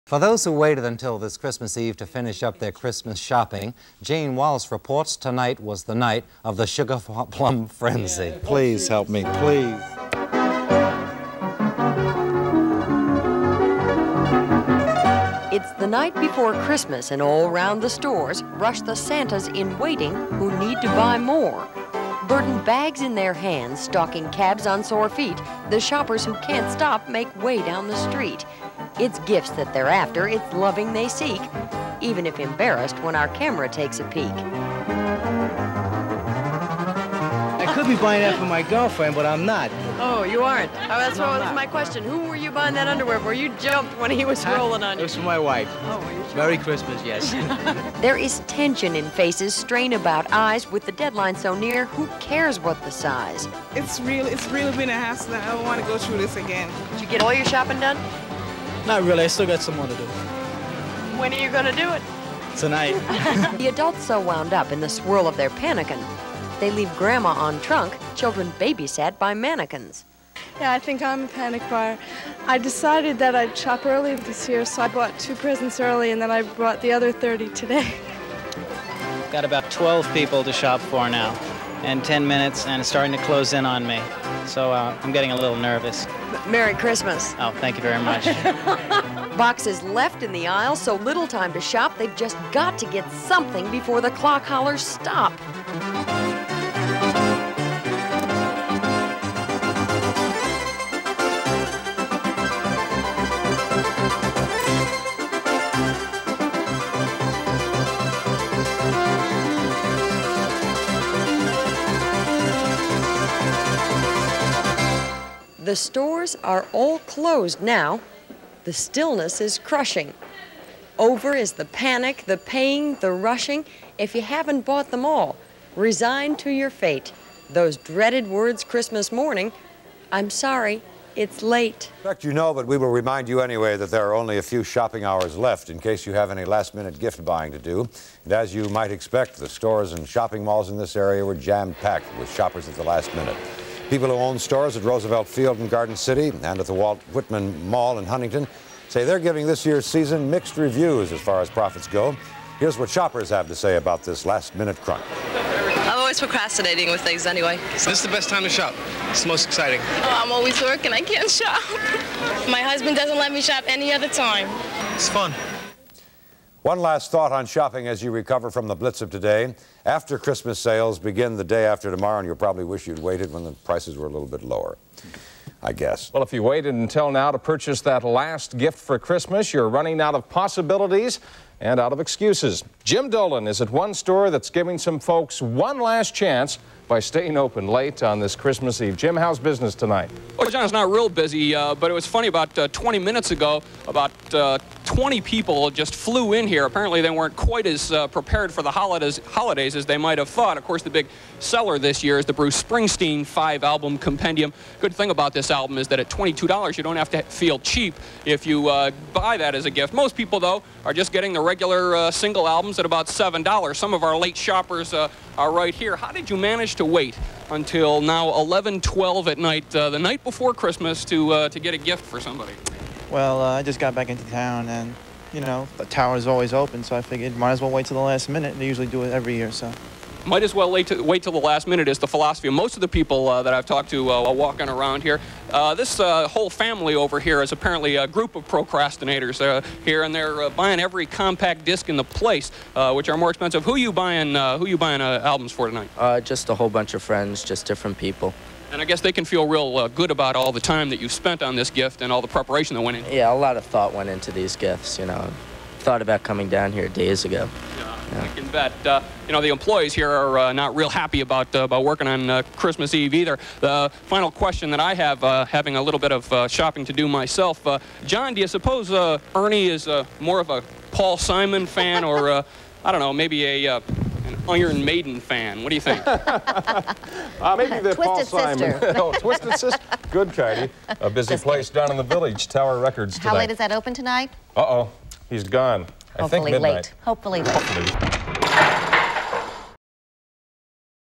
Sugar Plum Frenzy! See last-minute 1980s Christmas shoppers cope at New York malls
These reports from the Eyewitness News Vault are from 1989, 1984 and 1986.

NEW HERE? –